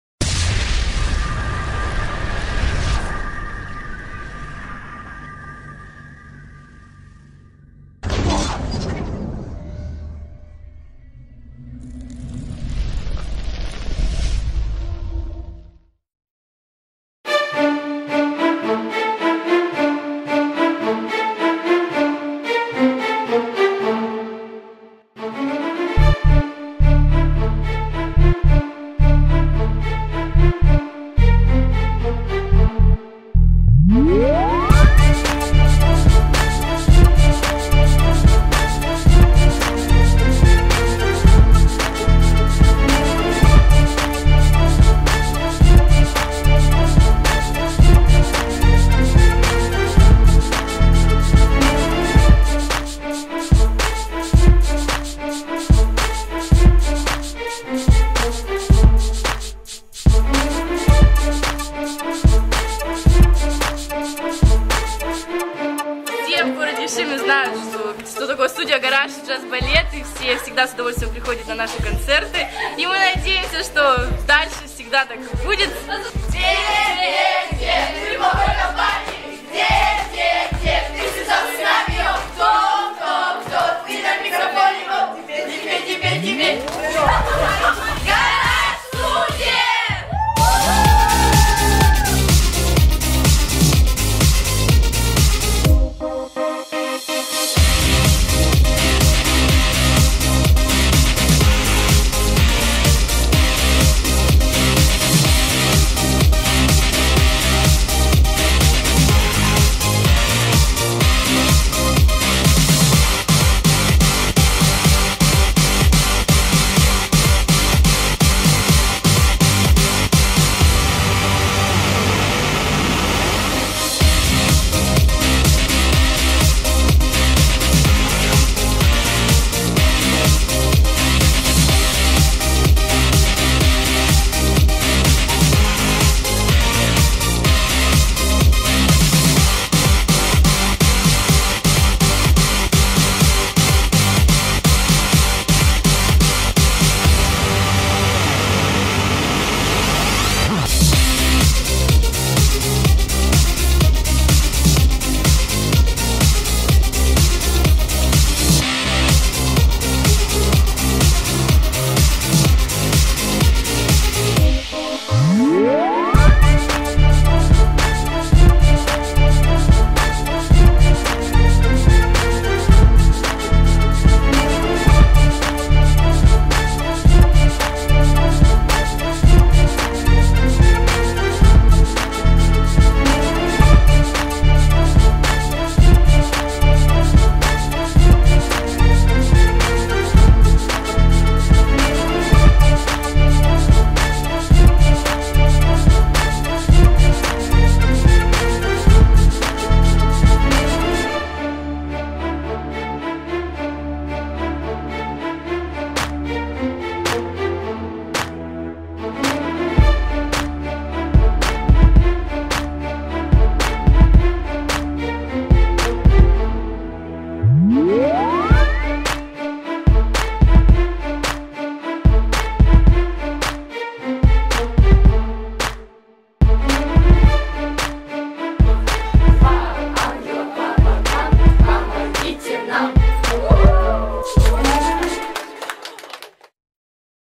так мы готовимся к концерту 2014
Концертная программа "5 МИРОВЫХ РЕЛИГИЙ" Ишимского JAZZ-BALLET'a и его студий GARA'J
24,25,26 мая 2014 год